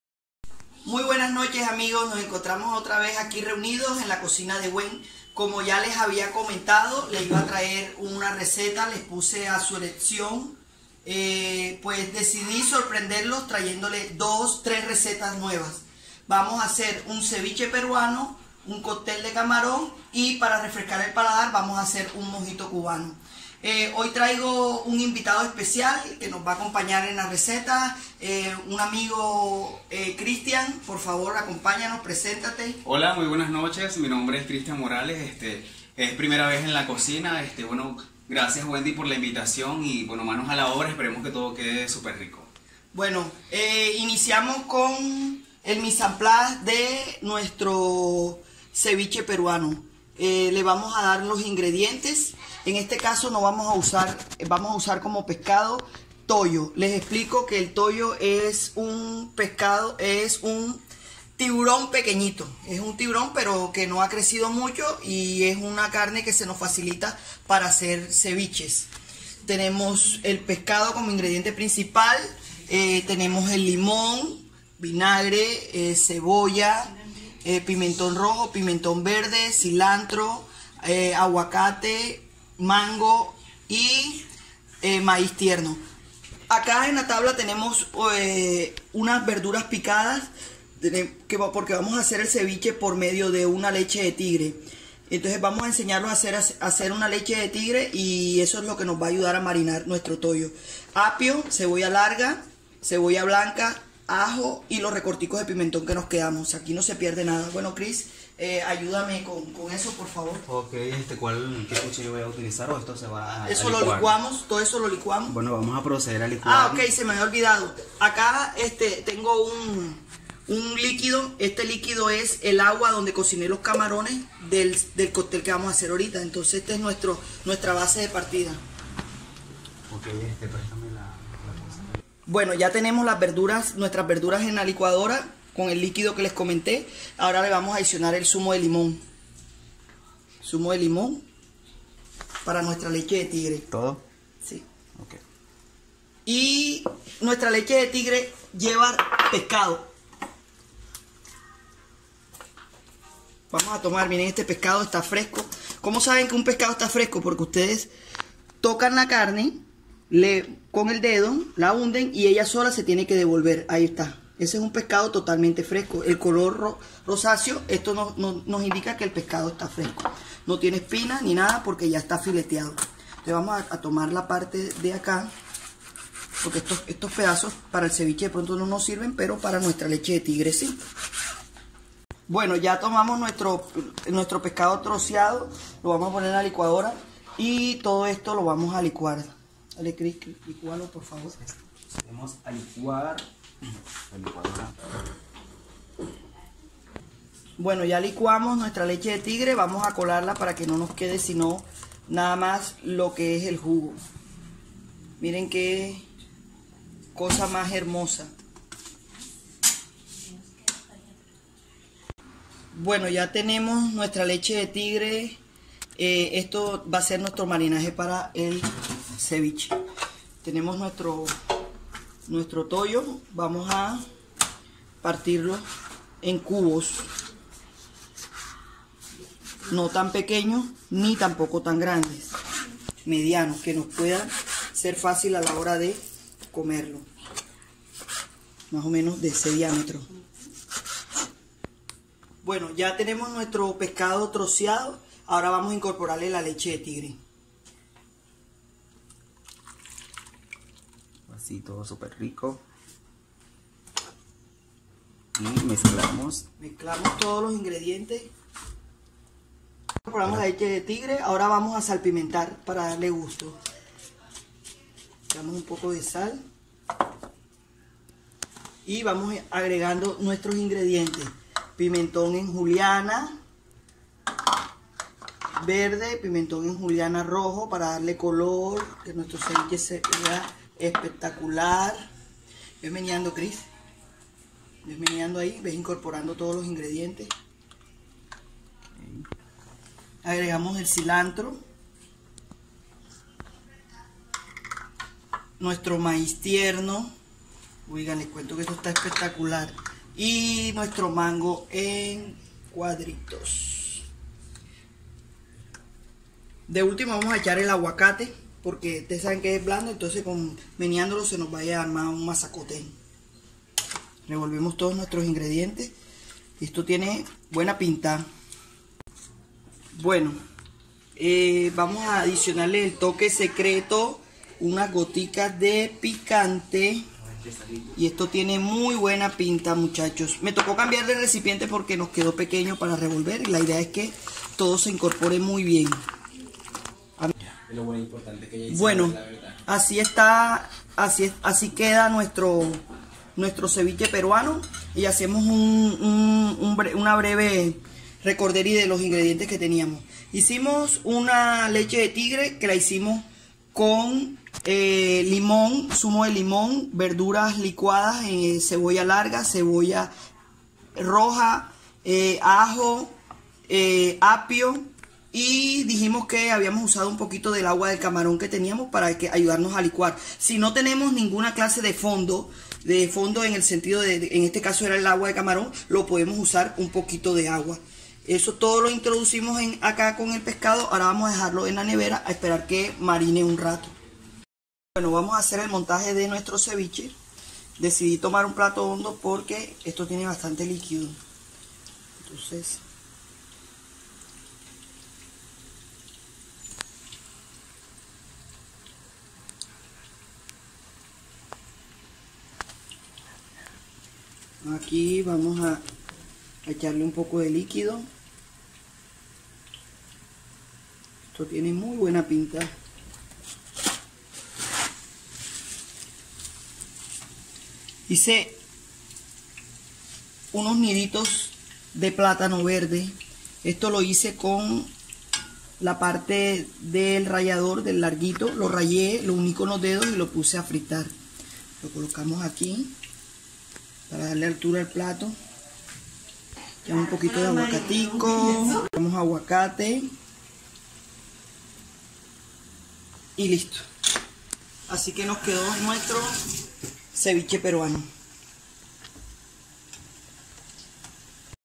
Ceviche peruano
Delicioso ceviche peruano con base de leche de Tigre, espero lo hagan y lo disfruten